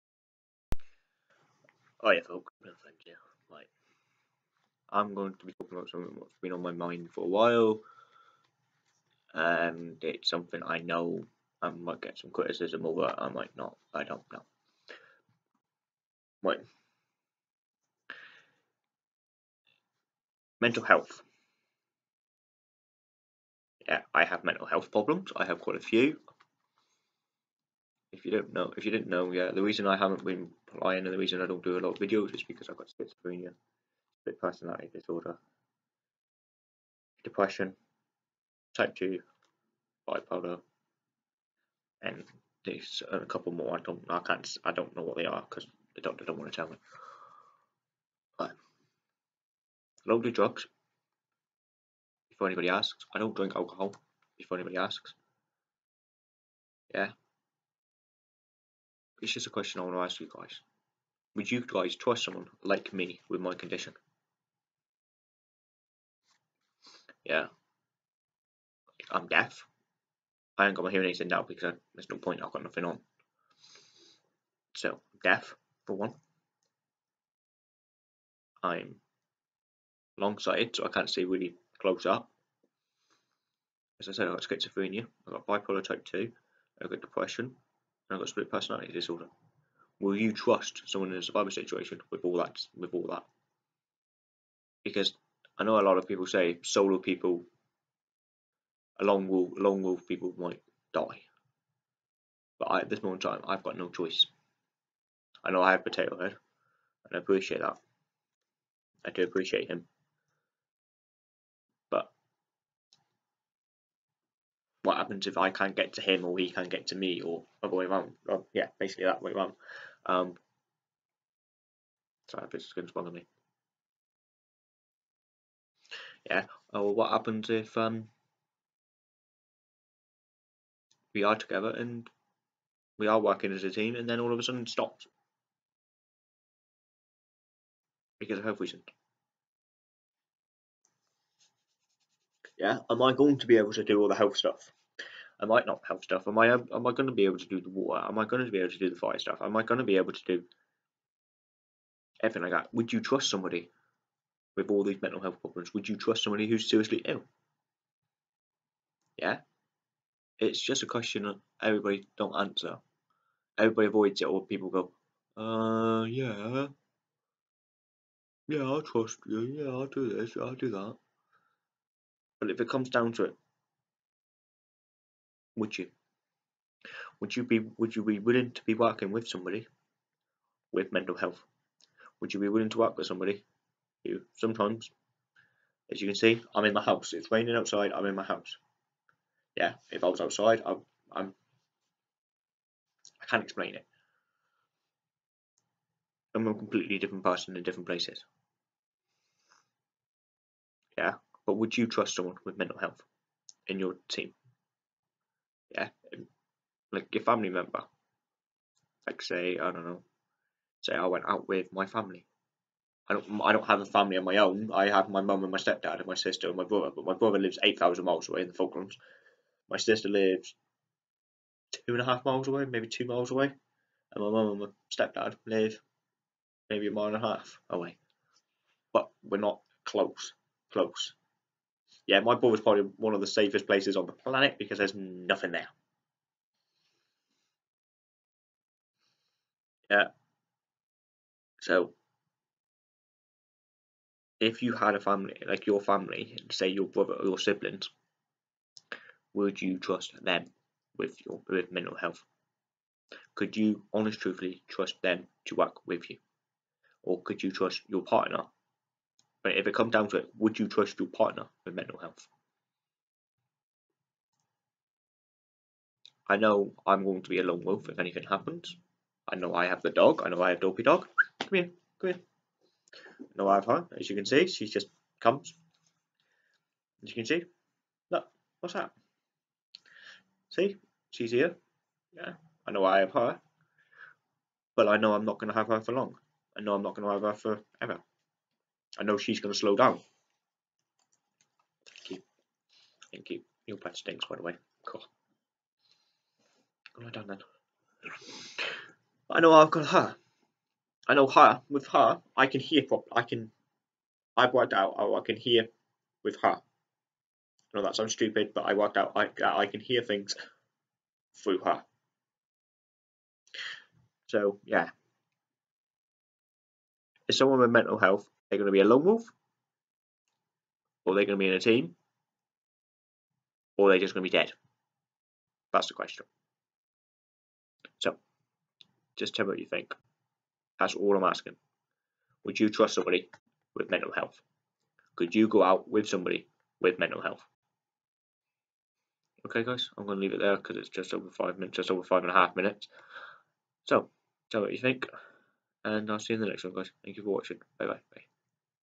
would you trust someone with mental health
would you trust someone with mental health when or if something happens and please tell me if you would or not and why